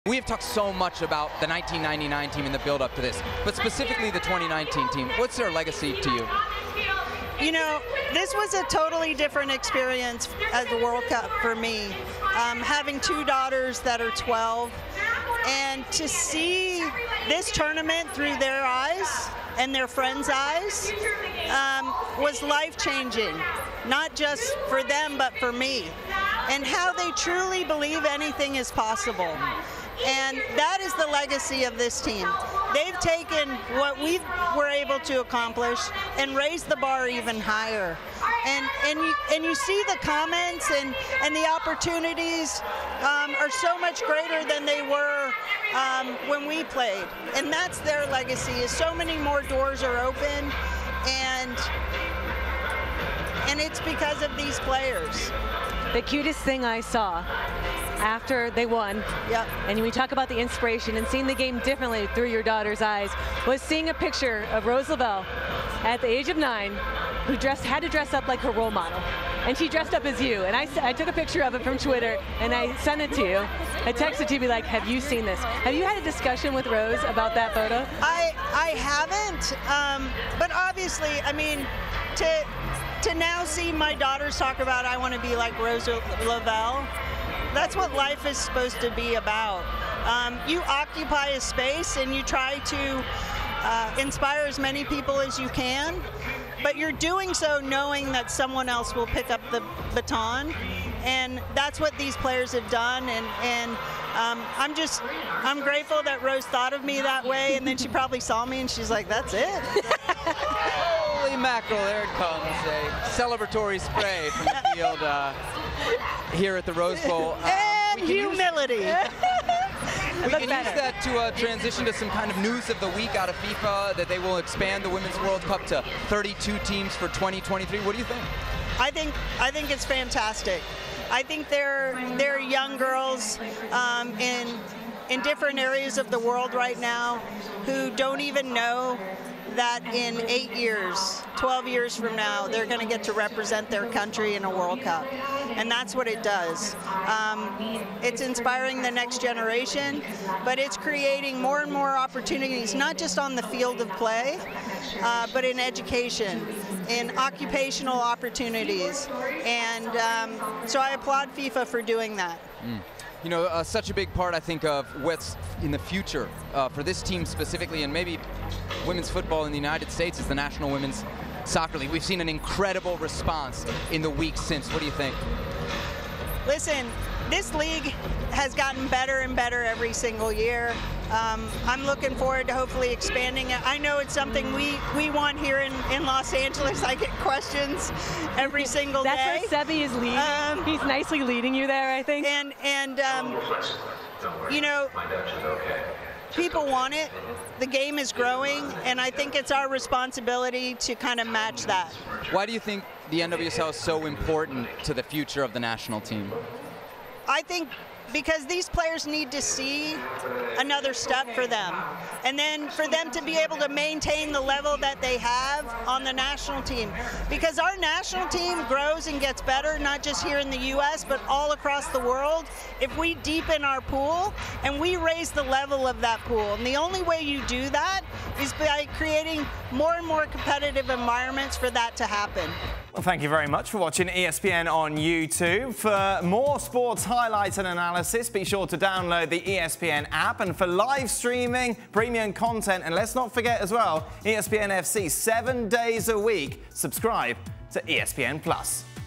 USWNT legend Mia Hamm on the 2019 team's legacy, World Cup expansion and the NWSL | ESPN FC
Two time FIFA Women’s World Cup champion Mia Hamm joins ESPN’s Sebastian Salazar and former teammate Kate Markgraf to discuss the United States women’s national team’s victory at the 2019 World Cup. Hamm, a USWNT legend, explains why watching the 2019 team was special for her and how the current team’s legacy raises the bar even beyond what she and the 1999 team accomplished. She also shares her thoughts on FIFA’s decisoin to expand the Women’s World Cup to 32 teams and the future of the NWSL, suggesting she’d like to bring a team to Los Angeles.